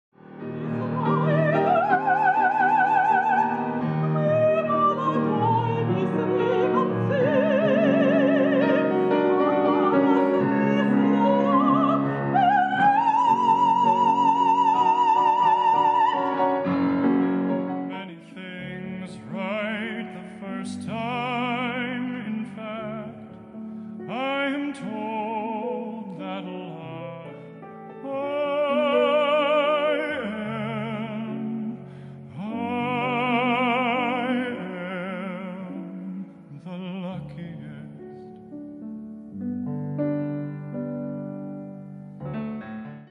20 CENTURY MUSIC - APRIL 6, 2019 - PROMO-VIDEO
An alluring evening of art songs by Sergey Rachmaninoff and Jazz songs from repertoire of Frank Sinatra.